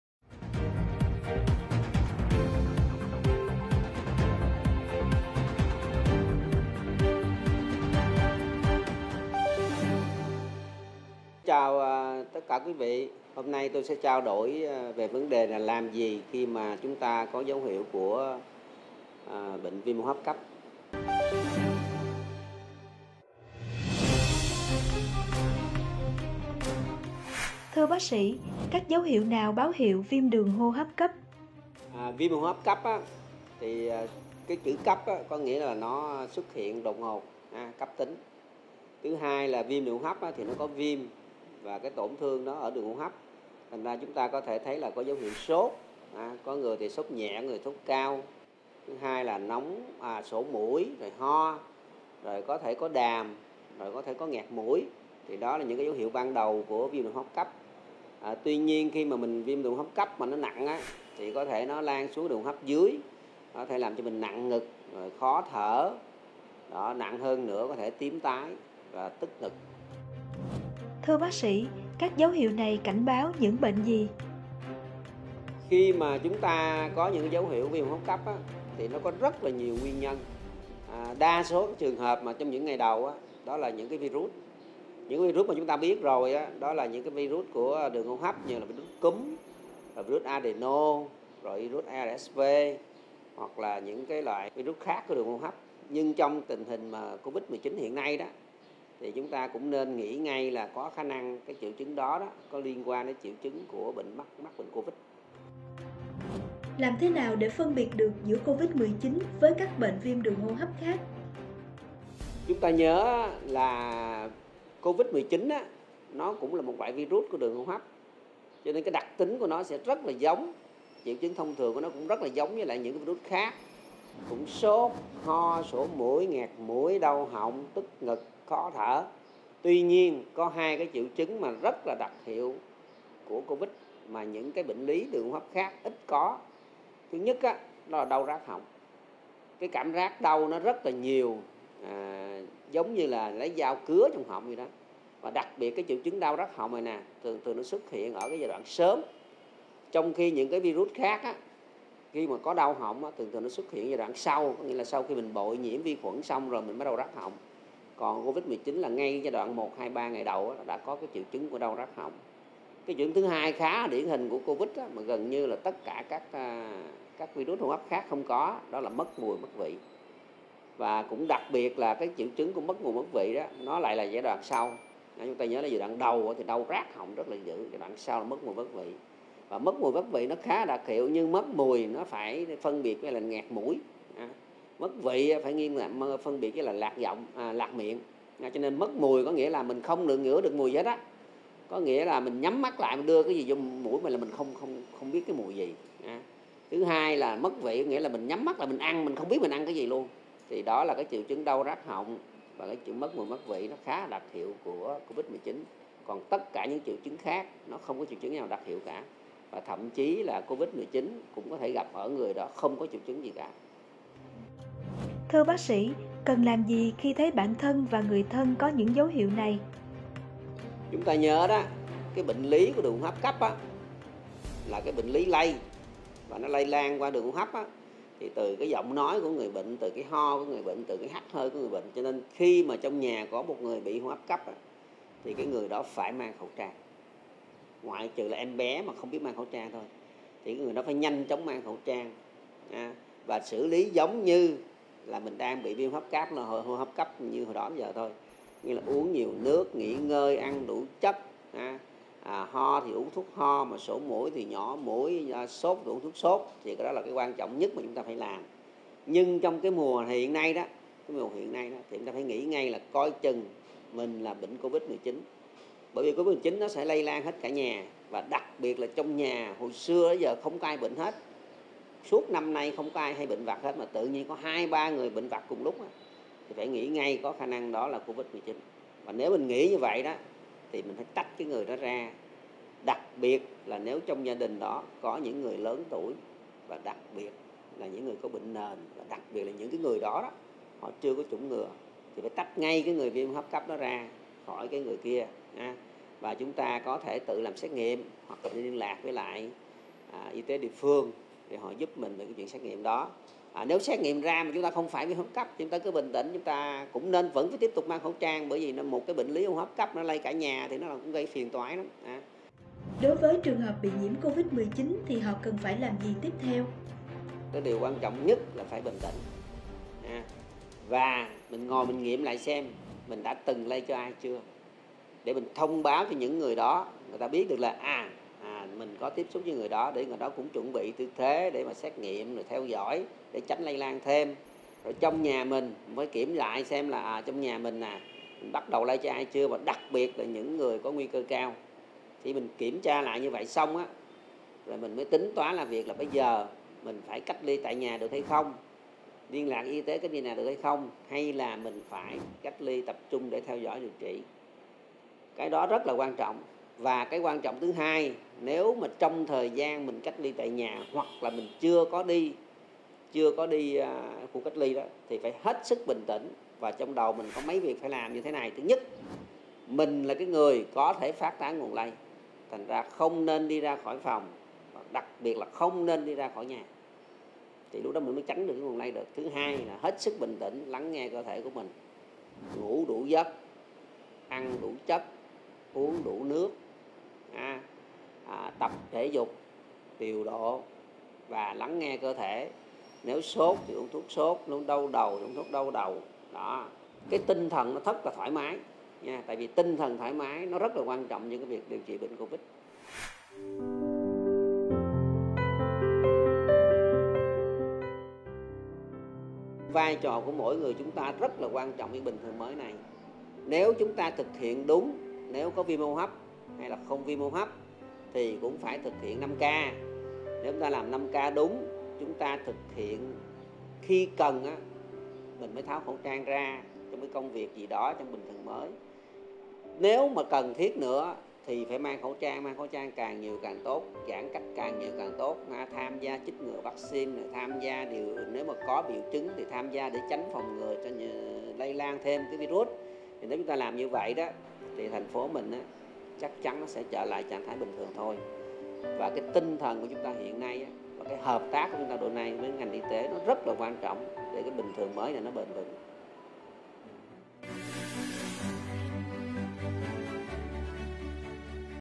Dấu hiệu nhận biết viêm hô hấp cấp
Viêm đường hô hấp cấp là một biểu hiện xuất hiện đột ngột, cấp tính và là biểu hiện của rất nhiều nguyên nhân trong đó đặc biệt có bệnh COVID-19. Vậy làm sao để phân biệt bệnh lý COVID-19 hay những bệnh hô hấp khác khi có biểu hiện viêm đường hô hấp cấp? Và những dấu hiệu nào báo hiệu có viêm đường hô hấp cấp?

Hãy cùng BS. Trương Hữu Khanh, Bệnh viện Nhi Đồng 1 TP.HCM tìm hiểu những vấn đề trên.

Nguồn : HCDC